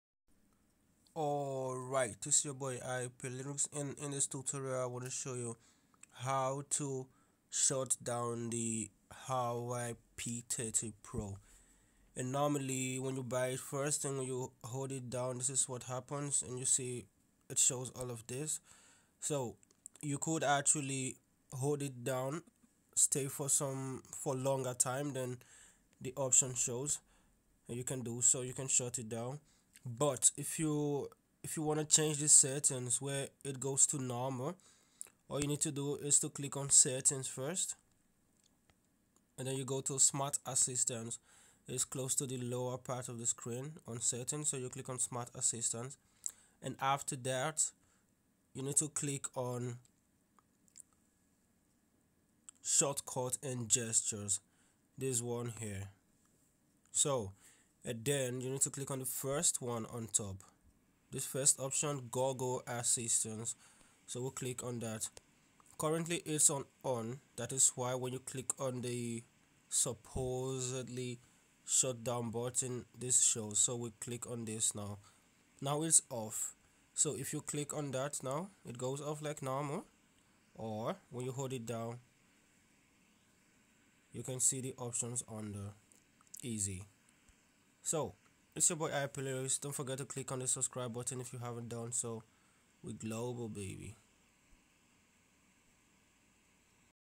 how to SHUT DOWN / RESTART HUAWEI P30 PRO (easy) •
I show you how to restart or shut down (power off, shut off) the Huawei P30 Pro.